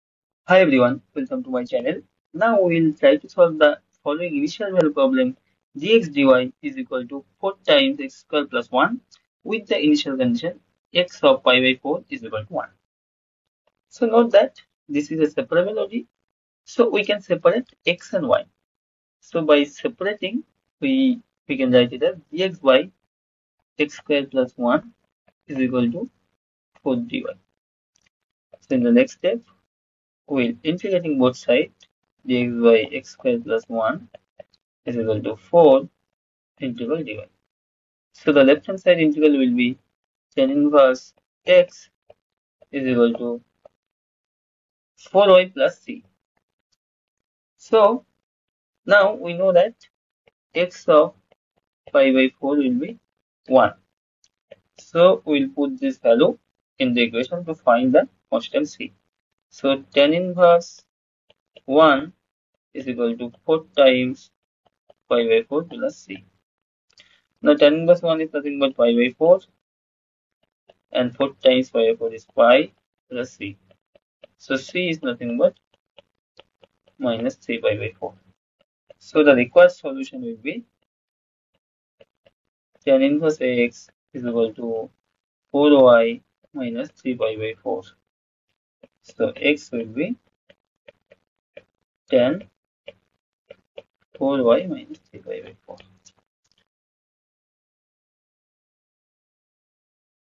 Solve: dx/dy= 4(x^2+1); x(pi/4)=1||Seperable Differential equation ||Initial value problem||ODE
In this video, we will solve the differential equation dx/dy=4(x^2+1); x(pi/4)=1 . This is an example of an initial value problem on seperable ODE. We will solve the given ODE by separation of variable method. After that we determine the constant by initial values. Here we try to provide step-by-step solution of solving seperable differential equation.